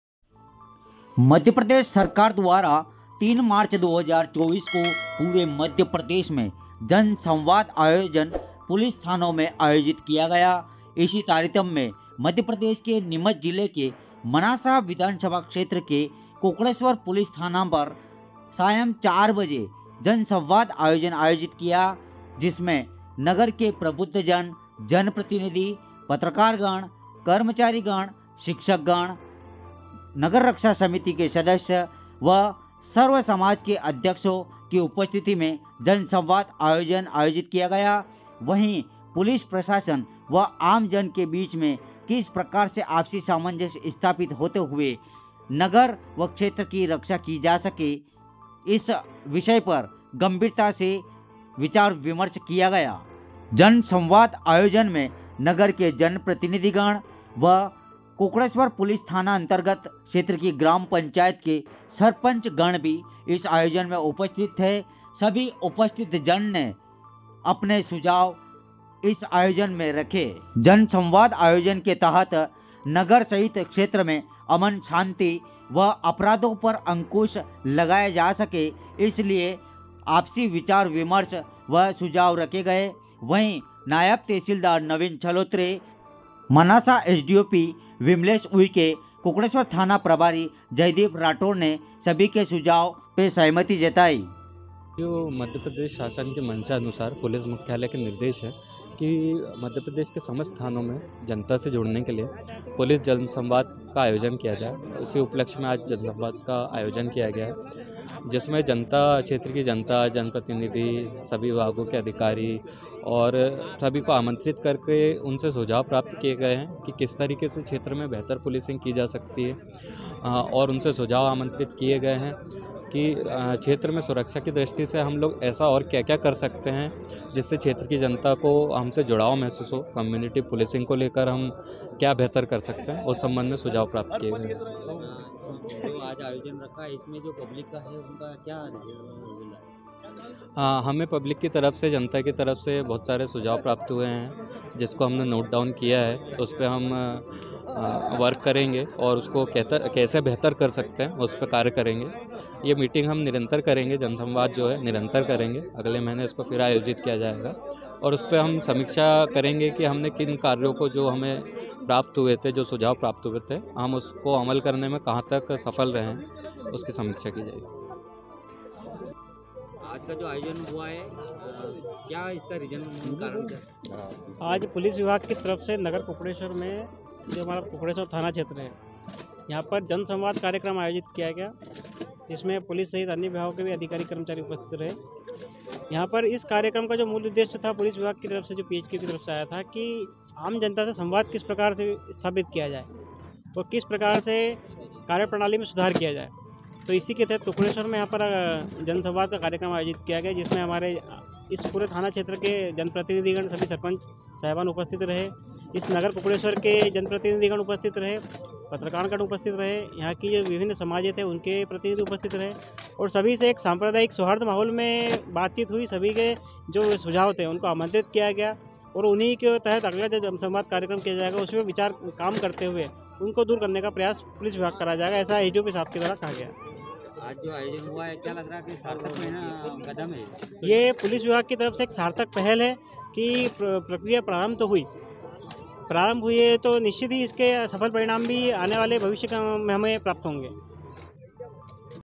अपराधो पर लगेगा अंकुश | CCTV कैमरे उतरेंगे मैदान में | पुलिस व आमजन की आपसी सहमति | जन संवाद | SDOP
@MP7NEWS अपराधो पर लगेगा अंकुश | CCTV कैमरे उतरेंगे मैदान में | पुलिस व आमजन की आपसी सहमति | जन संवाद | SDOPn

🤵🏼‍♂Gopaldas Bairagi
Chief Editor & Journalist - MP 7 NEWS